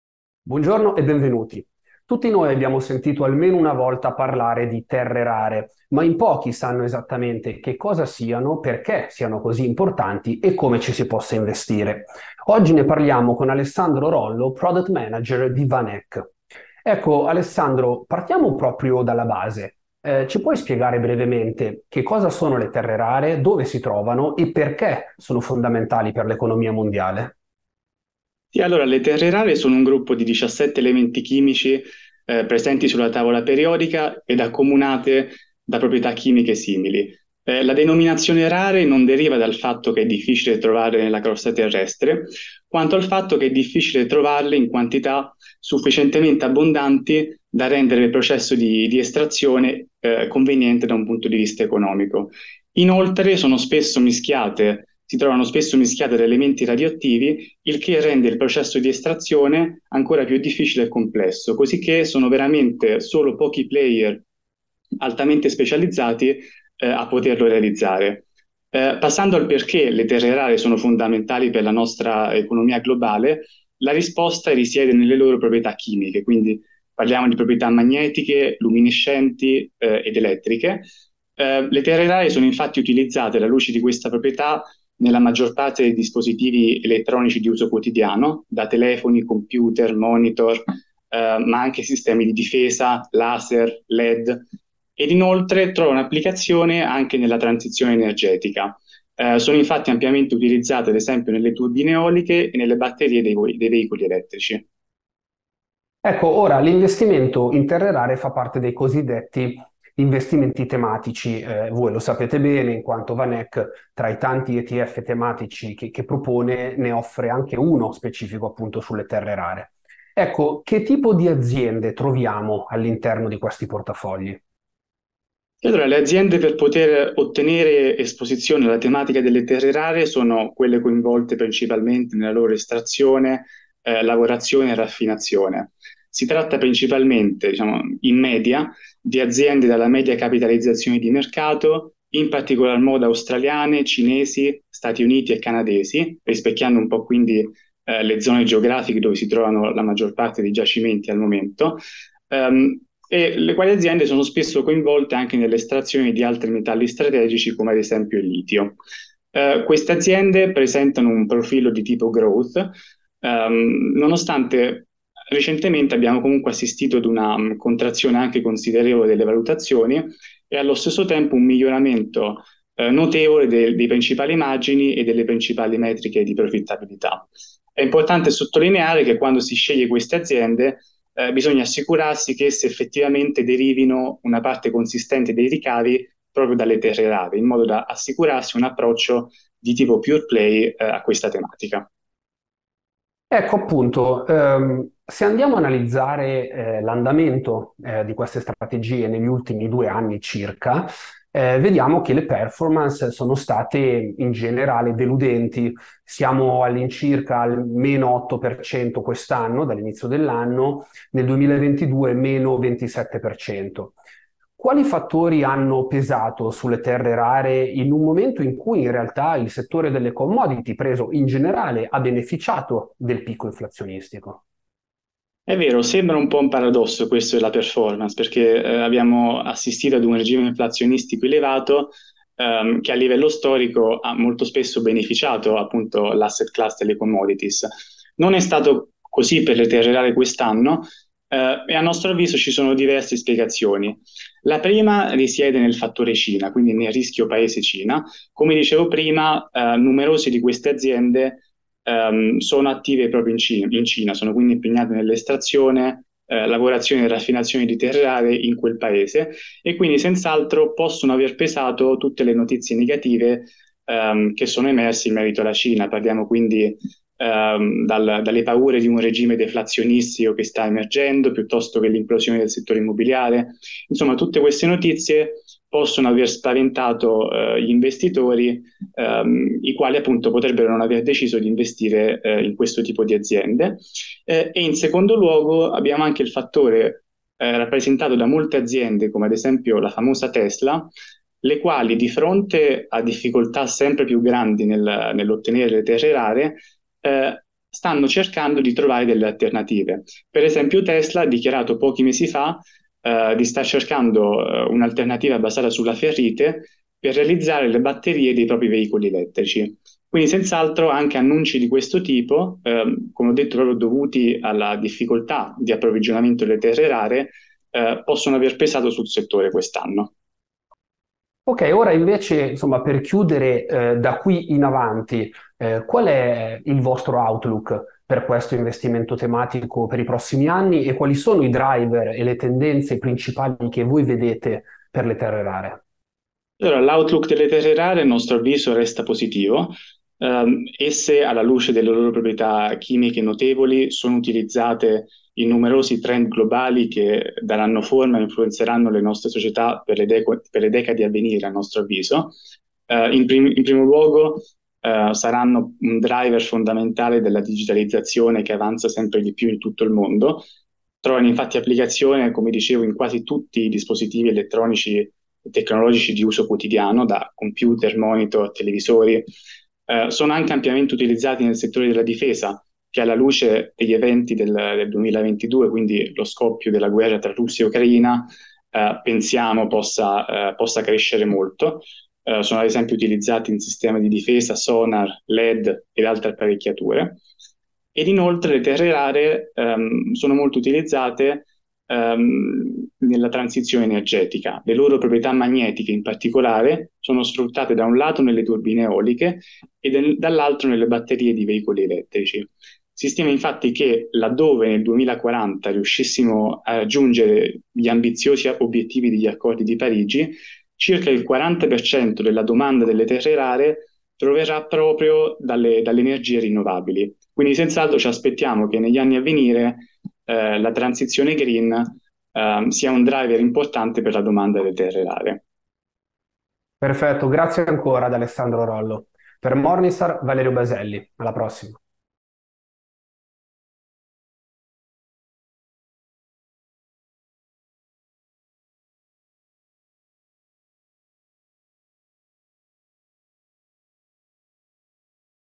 Quale futuro per le Terre rare?
Per Alessandro Rollo (VanEck) il fattore Cina (dove si trova il 70% delle miniere mondiali) ha pesato molto sulle performance delle aziende del settore, ma nel lungo periodo la domanda è destinata a crescere trainata da digitalizzazione e transizione energetica.